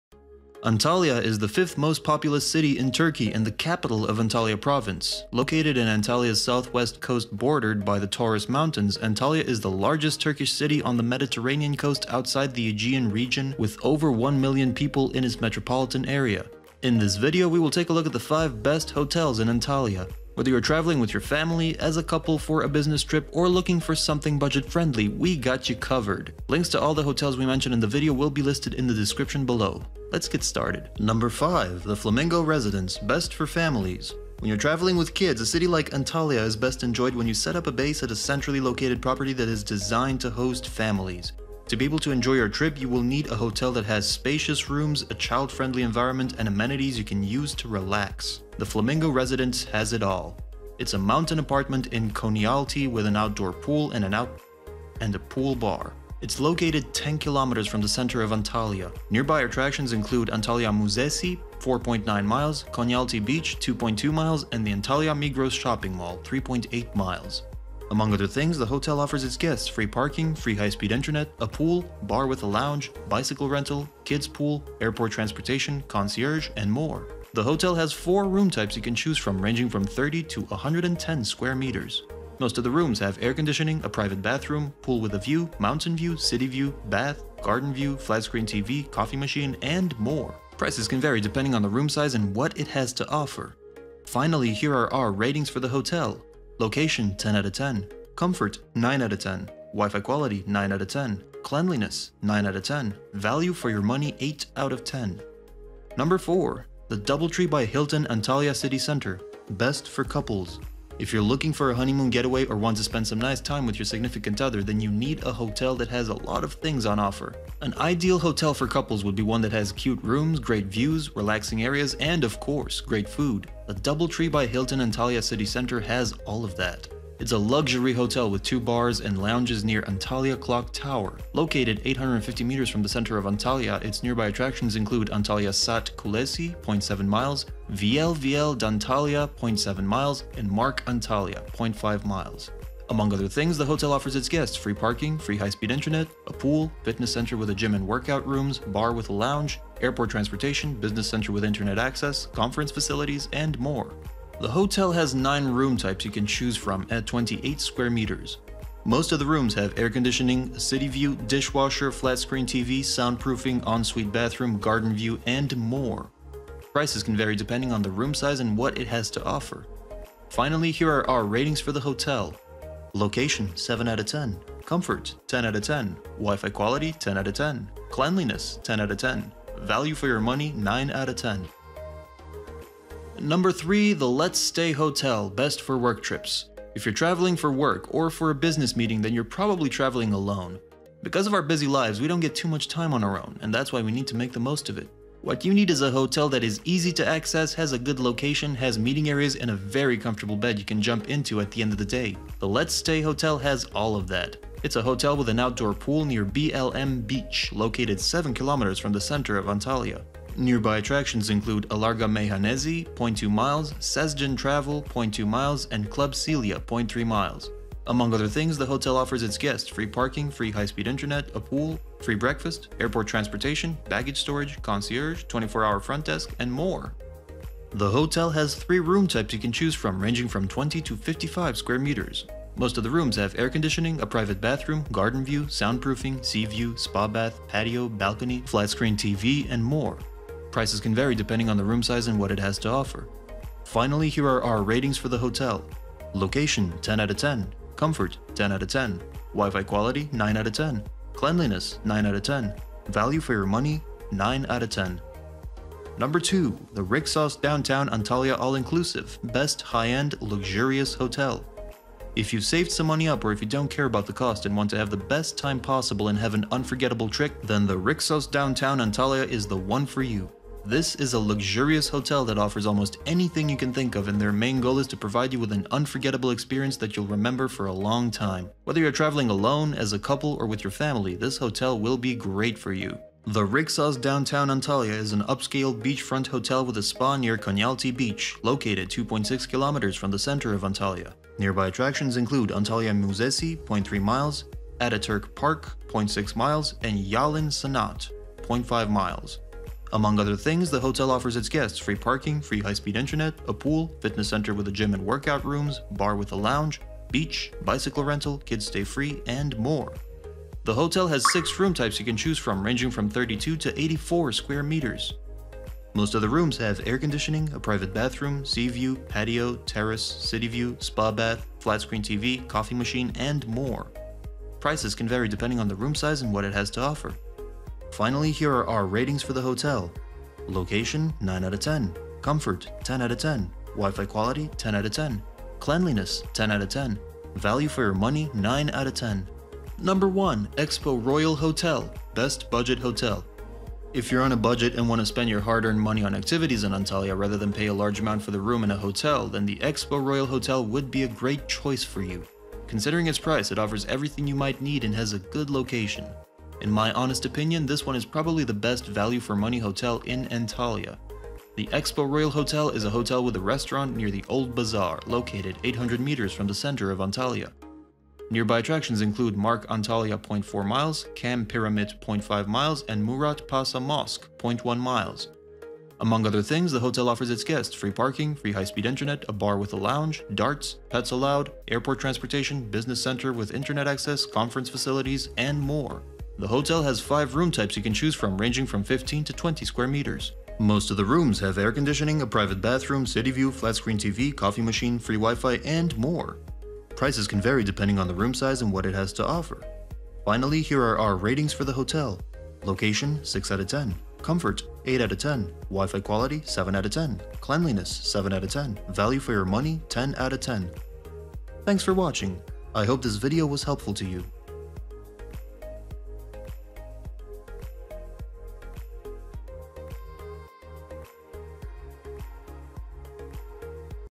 Best Hotels In Antalya, Turkey - For Families, Couples, Work Trips, Luxury & Budget
► Links to the best hotels in Antalya, Turkey we mentioned in this video:

► 4. DoubleTree By Hilton Antalya City Centre
► 2. Rixos Downtown Antalya All Inclusive

In this video, we listed the top 5 best hotels in  Antalya, Turkey. I made this list based on my personal opinion, and i tried to list them based on their price, quality, location and more. If you want to see the price and find out more information about these hotels, you can check out the links above.